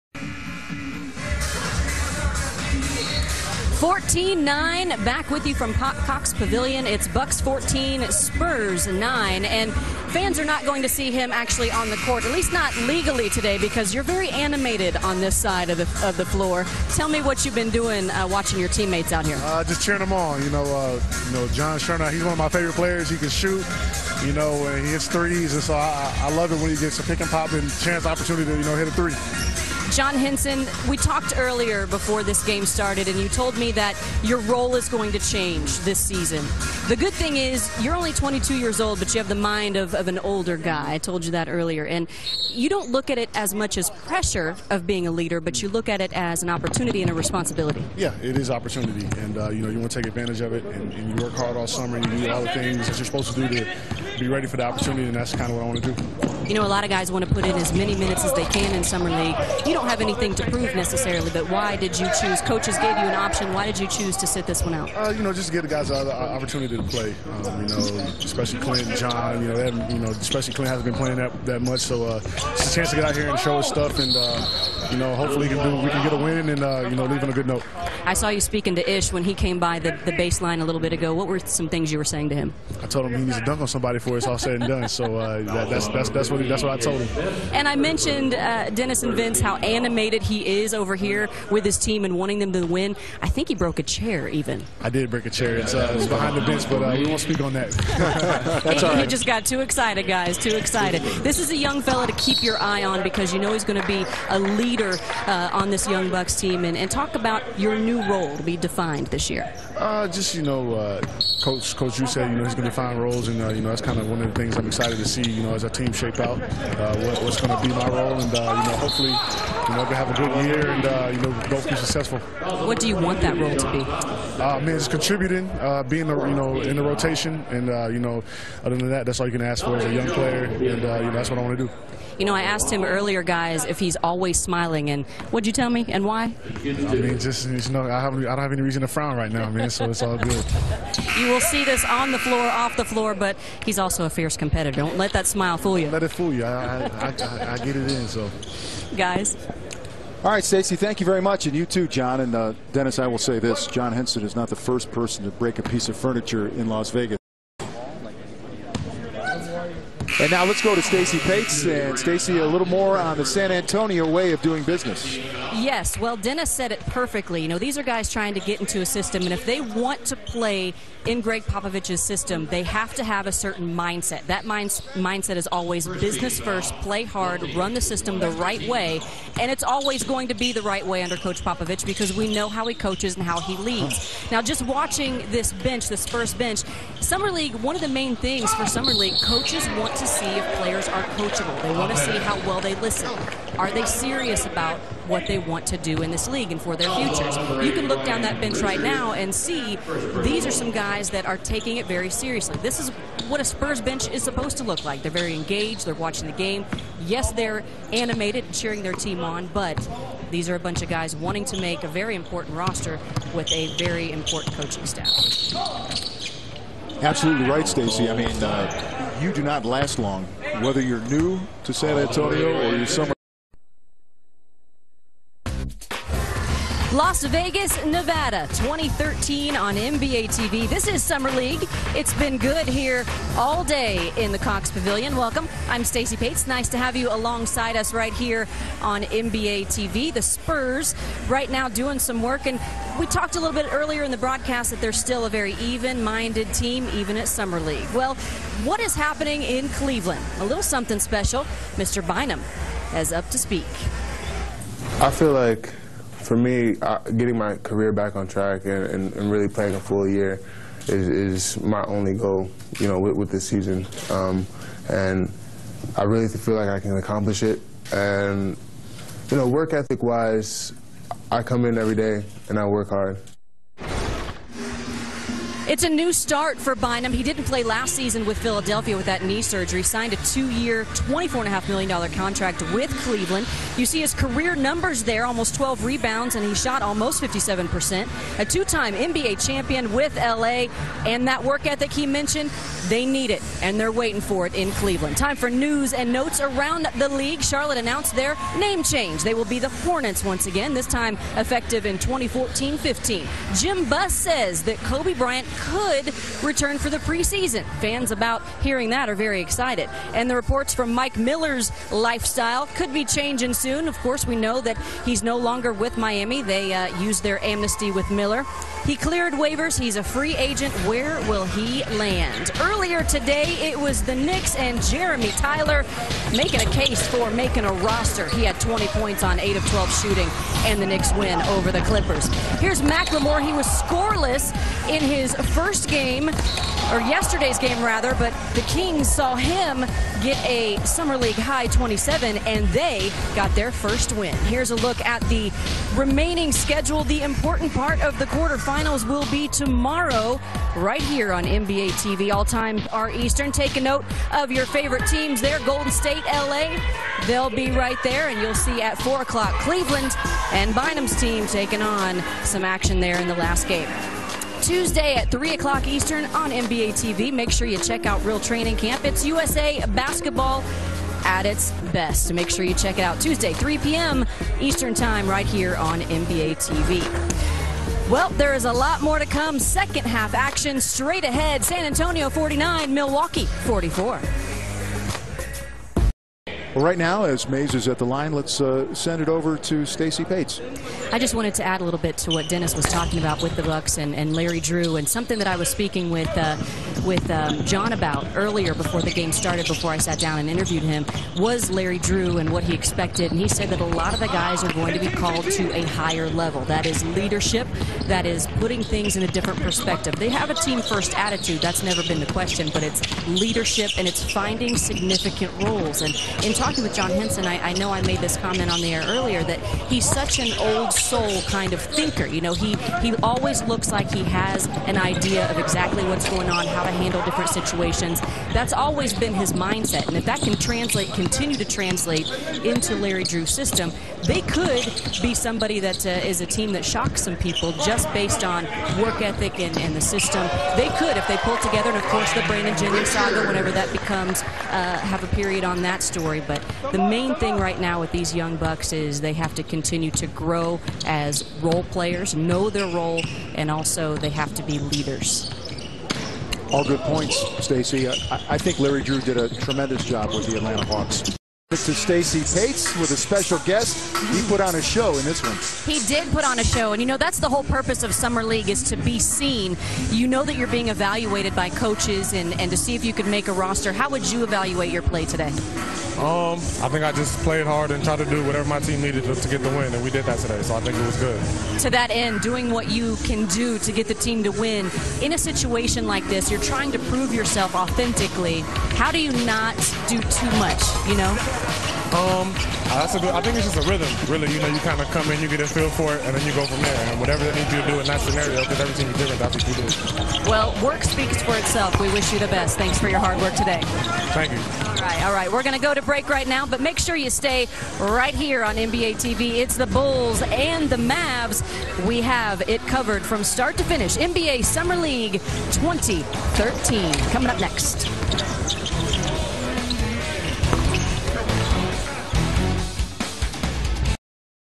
Stacy Paetz covering NBA Summer League game Spurs vs. Bucs
Stacy Paetz covers the NBA Summer League game between the Spurs and Bucs.